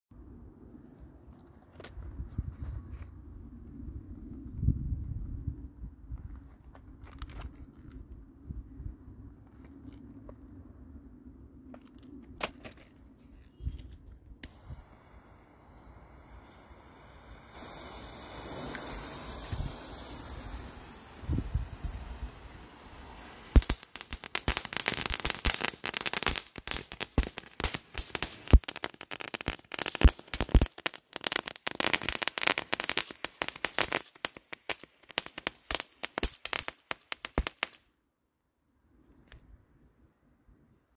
Lesli Crackling Carpet | Ich zünde eine Knallschnur | Echt laut | NICE firework string
Heute im Test: Lesli Crackling Carpet

* Kamera, Camcorder, Videoequipment:

*2x "3-Flammen-Zigarrenbrenner"

*2x BESTER BESTER Brenner (Sturmfeuerzeug)

* 2x Brenner (Sturmfeuerzeug)

KEEP WATCHING.